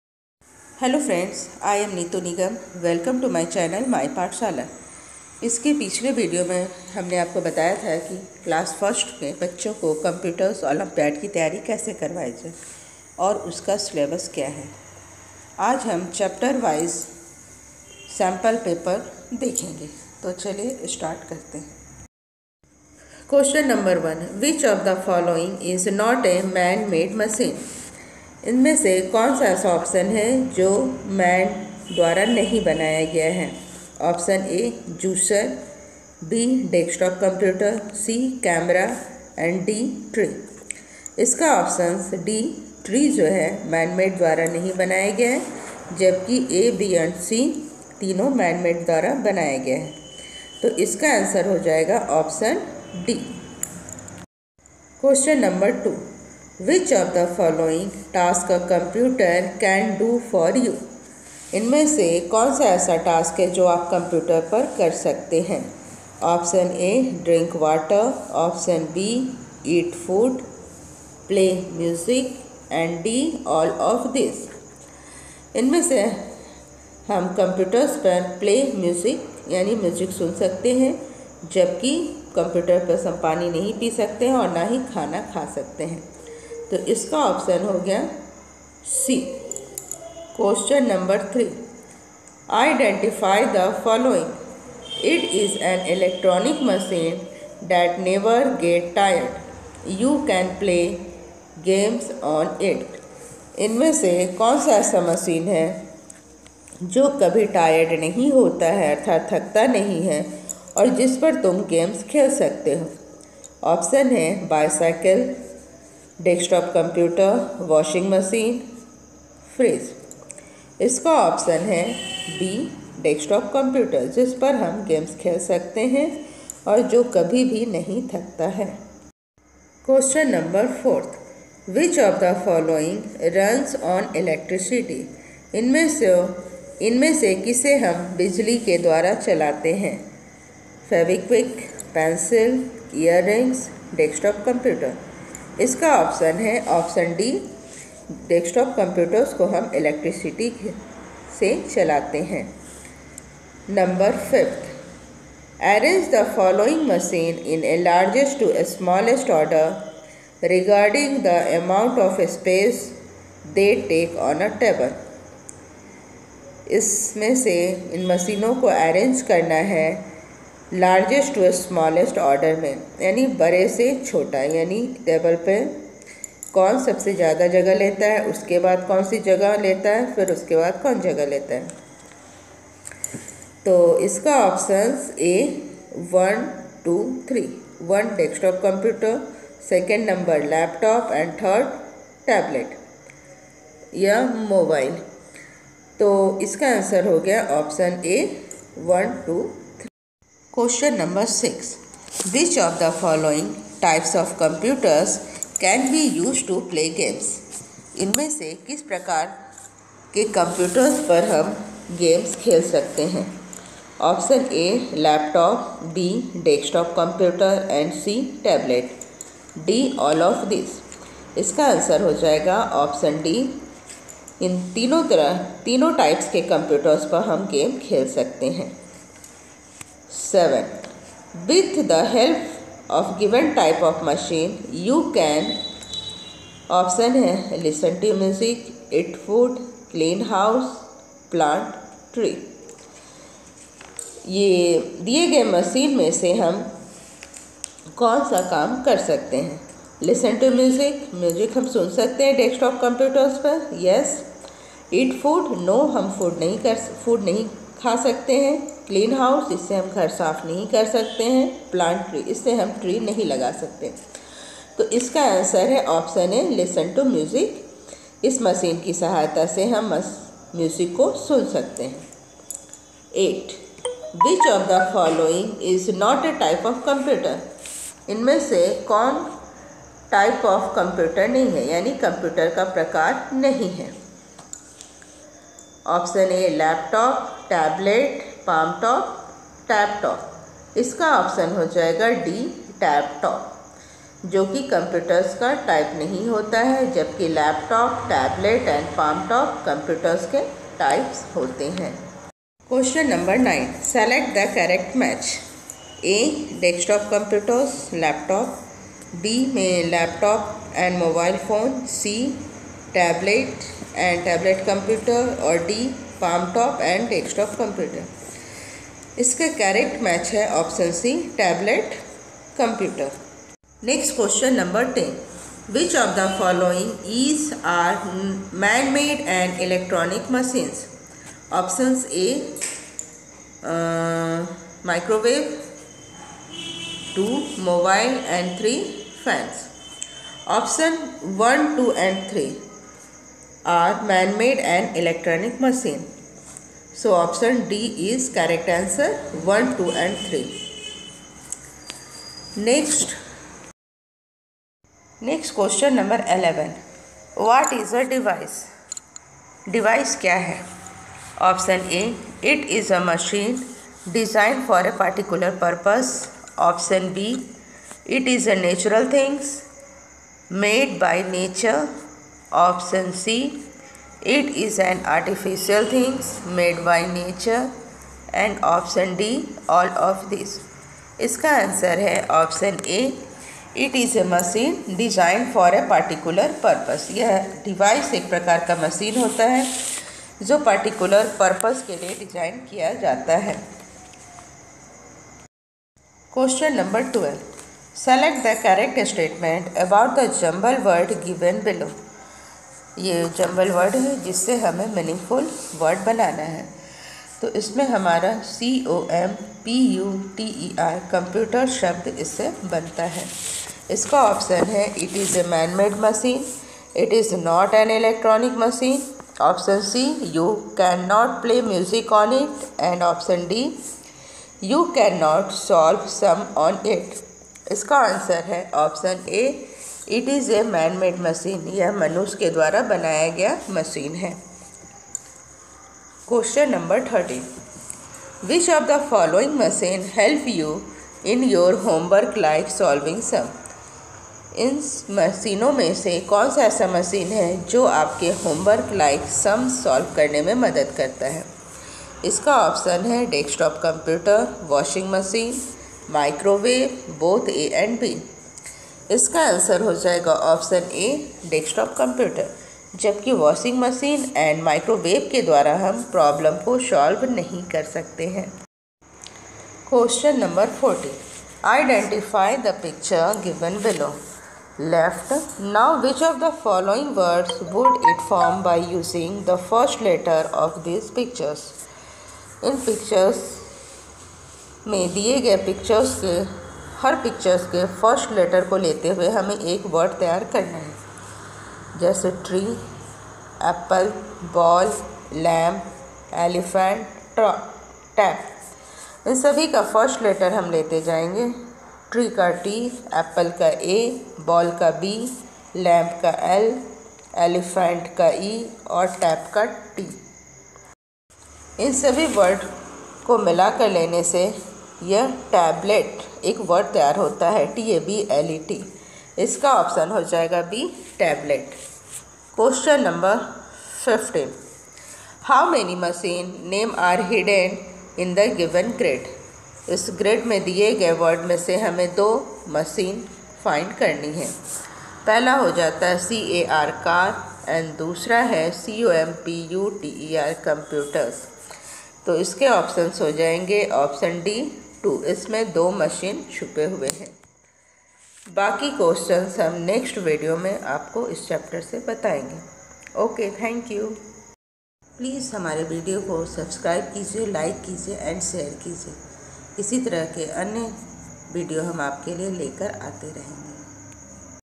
CLASS - 1 Introduction to computer Practice sheet
This Vedio I Explain  computer olympiad sample paper class 1
kis type ke Question pucha jata hai